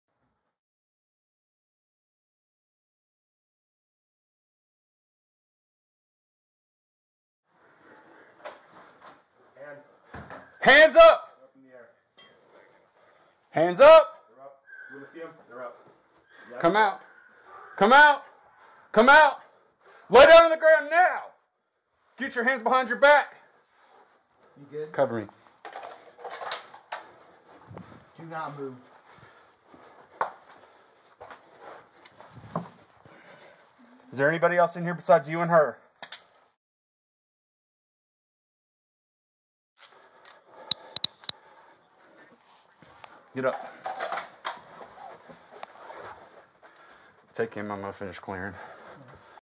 Police arrest of alleged kidnapper
Athens-Clarke County police recently arrested an alleged kidnapper who forced his ex-girlfriend into her apartment at knife point.